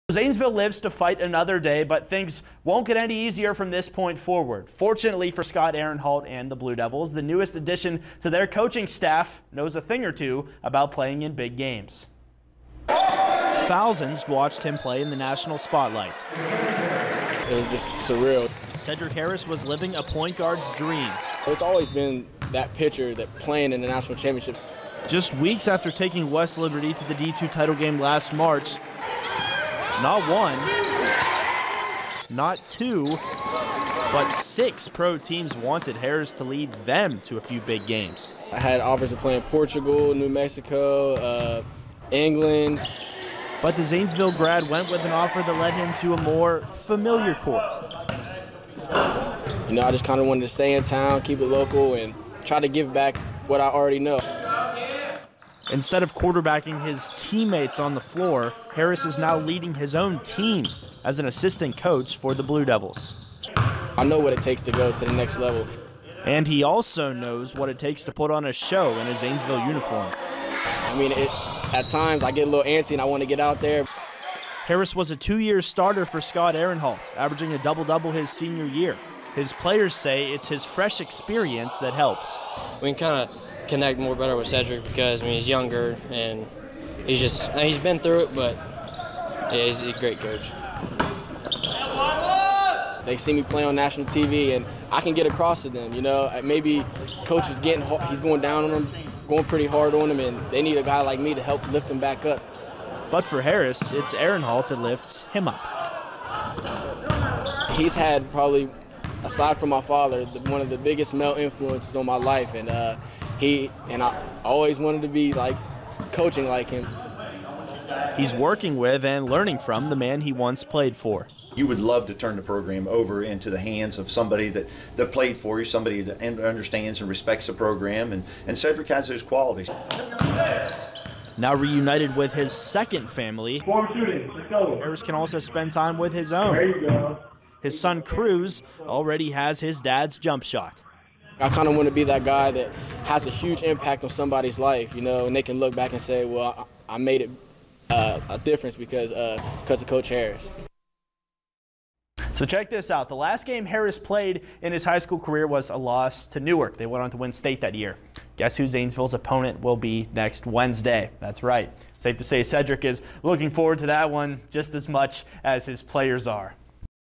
Cedric Harris PKG
The Zanesville Blue Devils have welcomed back a familiar face as an assistant coach.

Pat Chiesa is the sports director at WHIZ Media Group in Zanesville, Ohio.